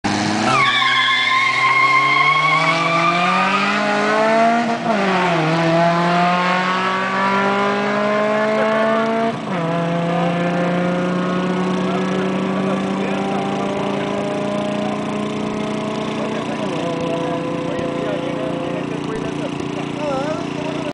DRAGTEAM civic eg ls/vtec vs chysler STRATUS 2.4 TURBO
arrancones todos los domingos de cada 15 dias en el autodromo internacional fco. villa en la ciudad de chihuahua, carrera entre un honda civic ls/vtec ( EL RAYO MACKQUEEN ) de DRAGTEAM vs un stratus 2.4 turbo .tony araujo.